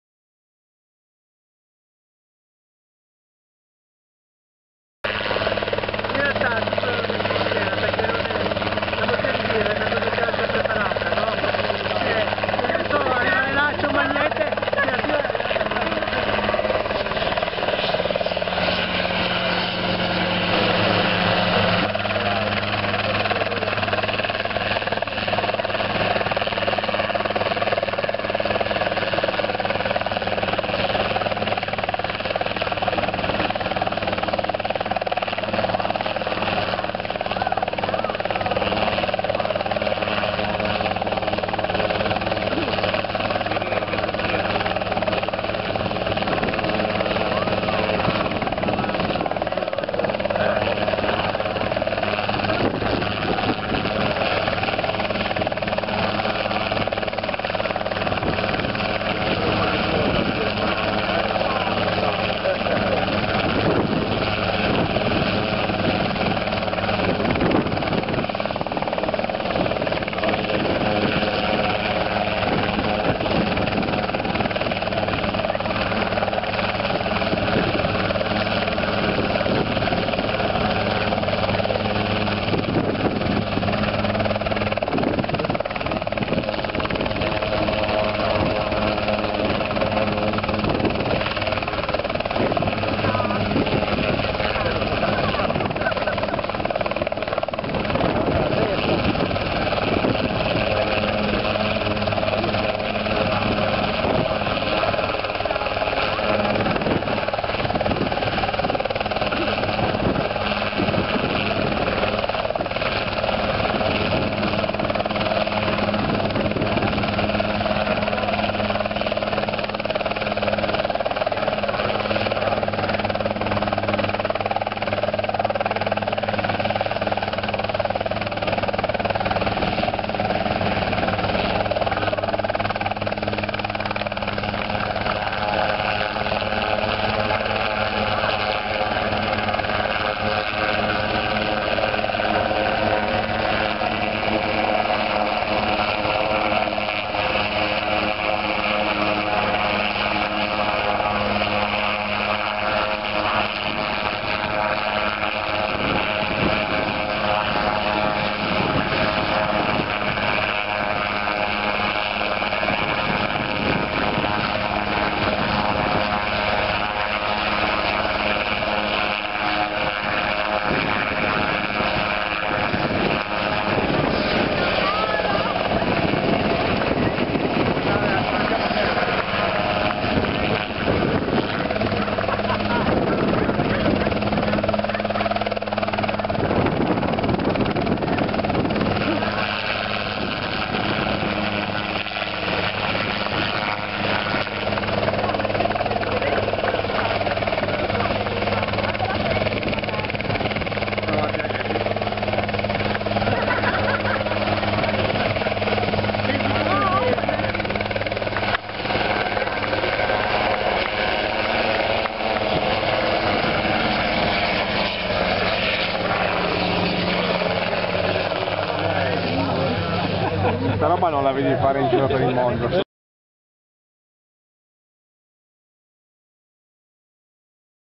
How to open a bottle of beer by a helicopter?
Precision piloting skill demonstration by Igi Barbero on a Helisport CH-77 ultralight helicopter. Demonstration includes opening a bottle of beer by a bottle opener mounted on the sled.
Place: Pianezza airfield, Piemonte, Italy. Date: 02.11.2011

Dimostrazione dell pilotaggio di precisione con un Helisport CH-77 elicottero ultraleggero. Pilota - Igi Barbero. Posto - campo volo Pianezza, durante la festa di volo il 2 ottobre 2011.